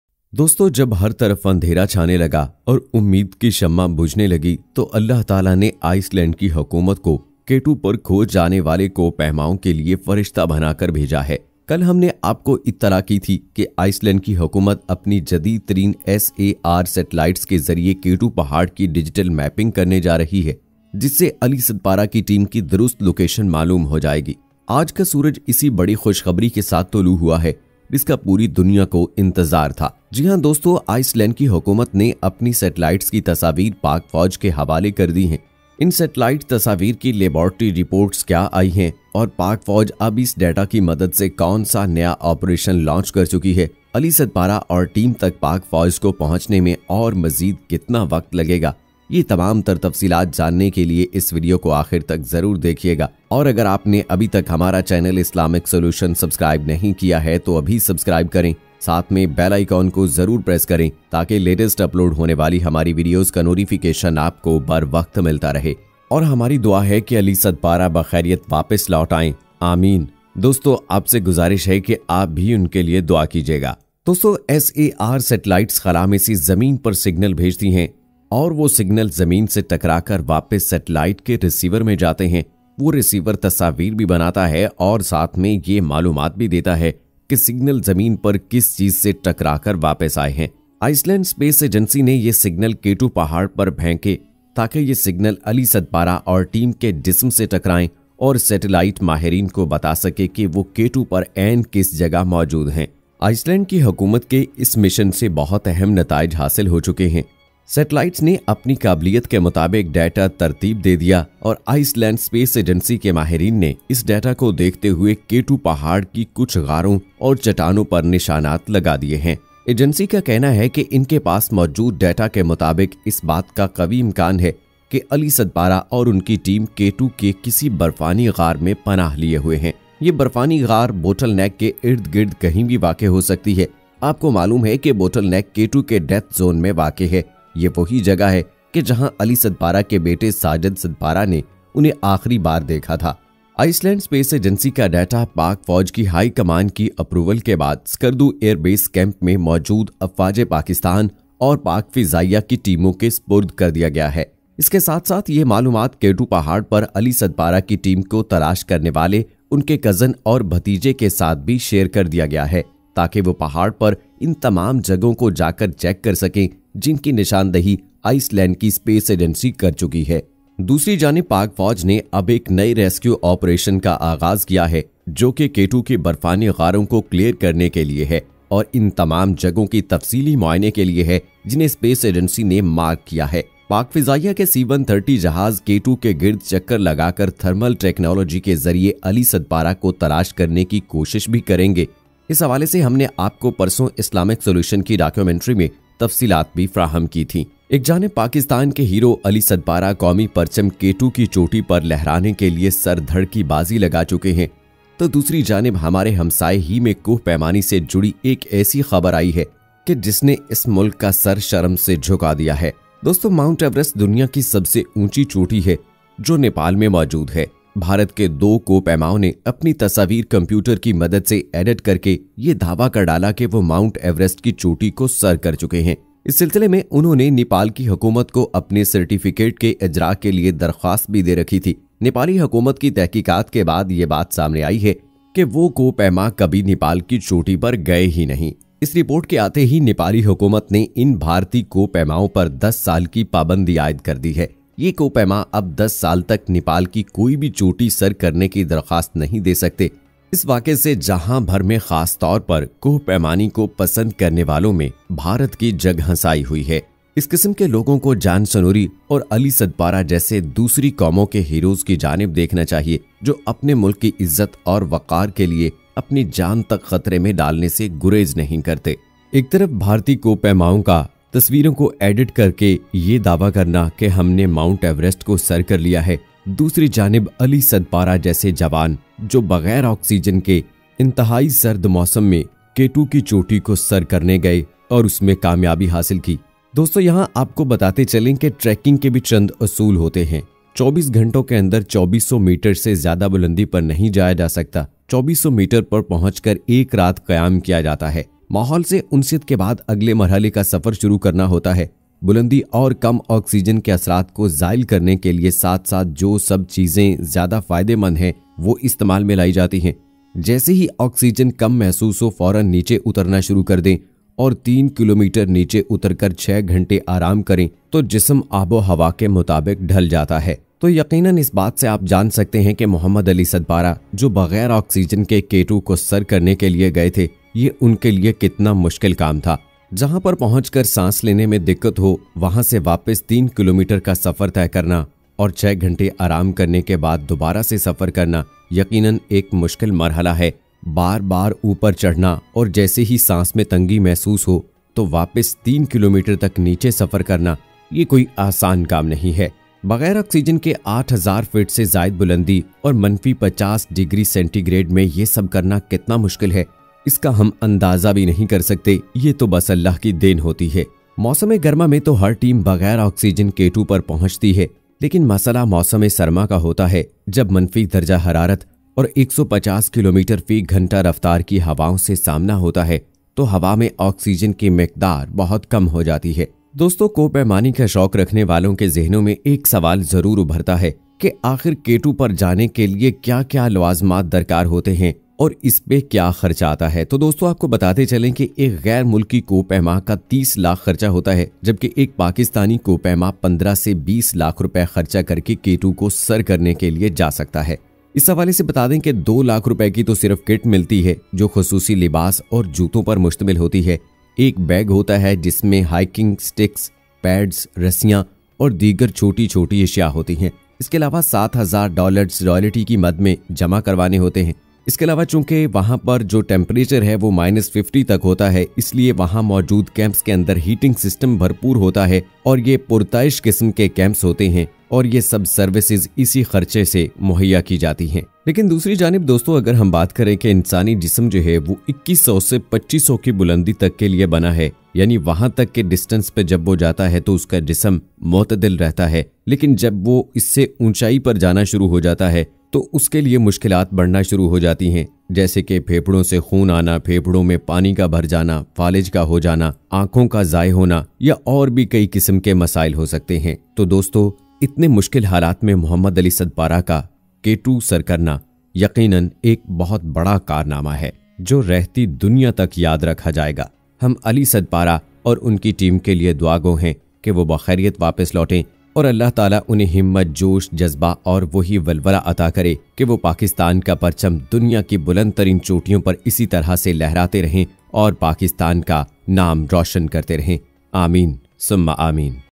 Iceland Space Agency released images of Actual Location of Ali Sadpara
Icelandic Aerospace Agency released satellite images of K2. Images were taken by the Icelandic Aerospace Agency during the search operation of the Snorri-Sadpara-Mohr team. John Snorri from Iceland, Muhamad Ali Sadpara from Pakistan, and Juan Pablo Mohr from Chile gone missing on the K2 Winter expedition when they lost contact with Sajid Ali Sadpara. Sajid last time saw them climbing up at bottleneck(8200m).

JazakAllah Khair